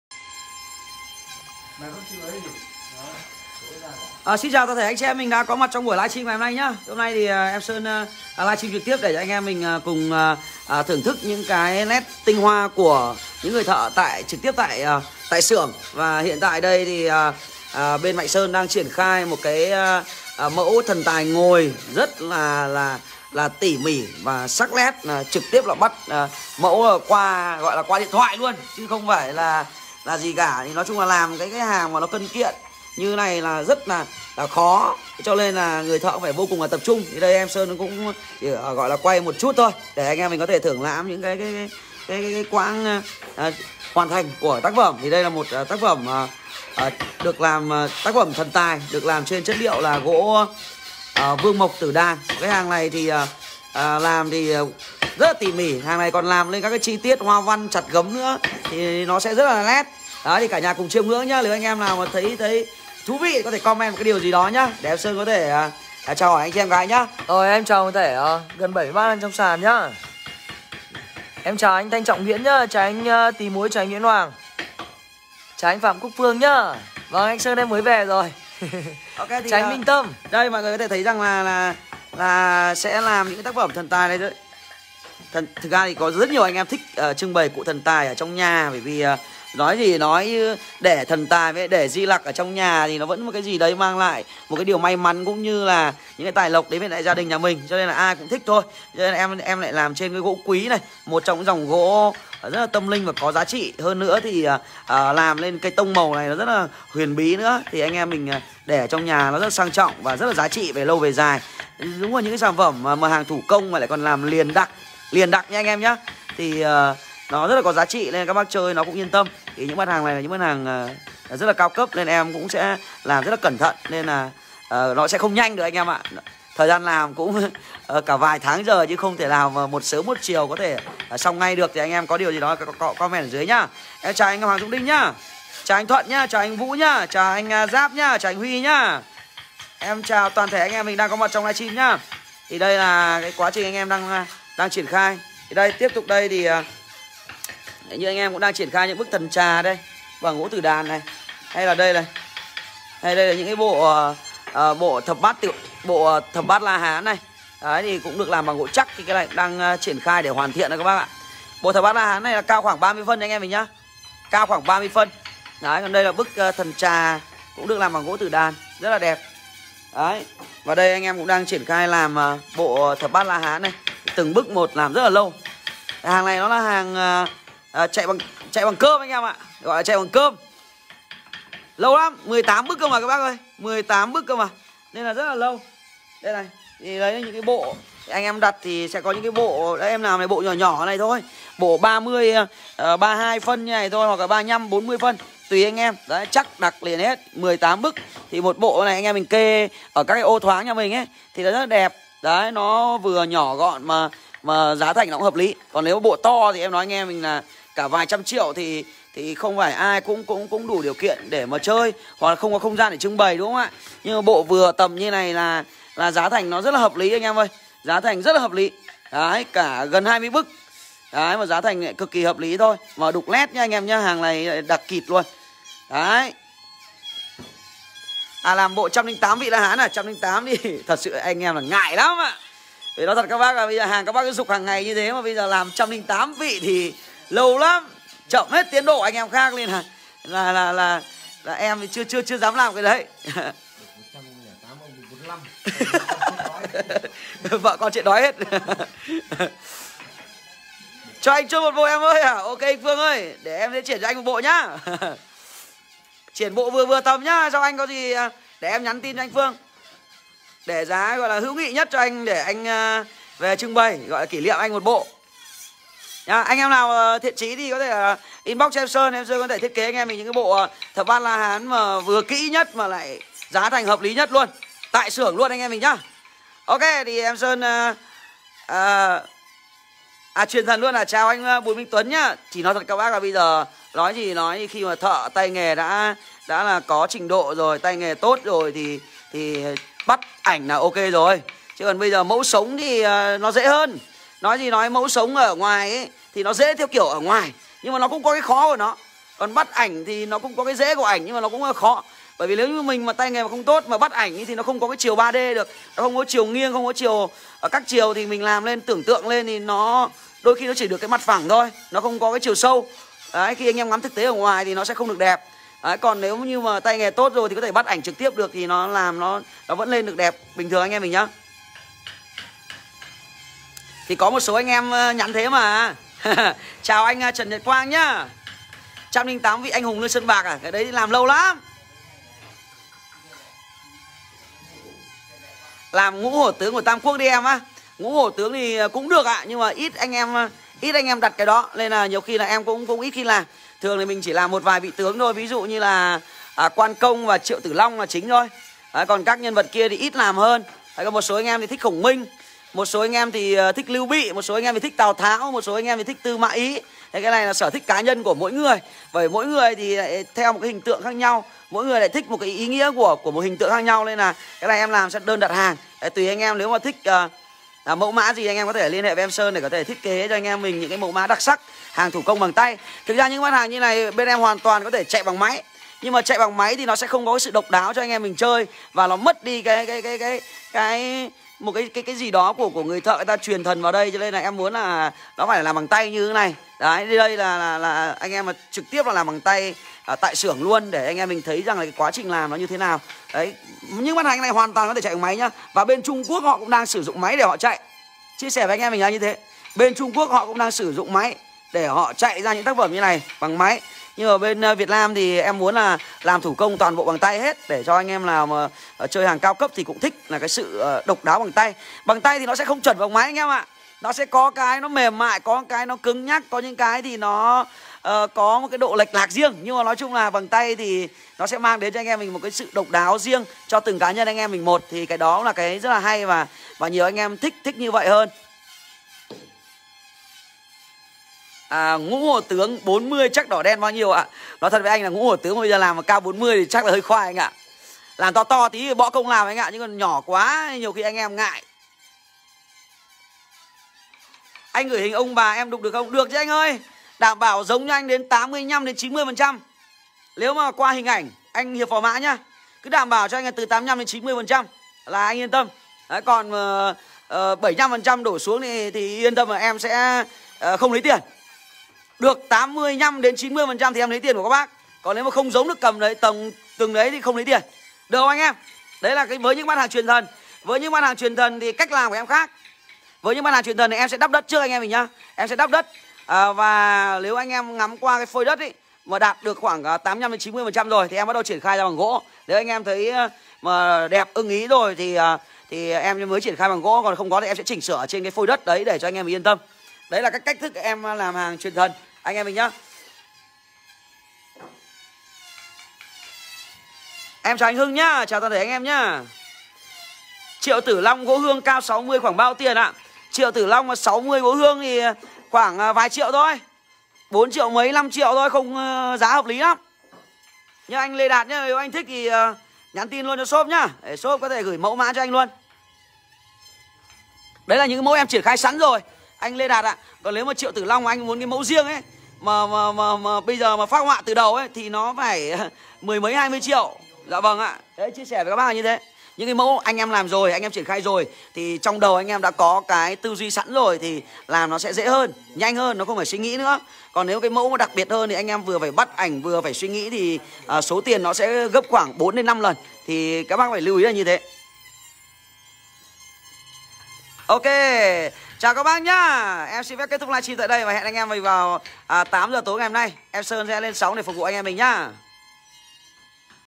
Khám Phá Xưởng Chạm Khắc Tượng Gỗ Thần Tài Tại Đồ Gỗ Mạnh Sơn
- Tượng Gỗ Phật Di Lạc Ngũ Phúc Thuần Nghê Nu Hương
🚩Địa Chỉ Cở Sở 1: 125A Làng Nghề Thiết Úng - Vân Hà - Đông Anh - Hà Nội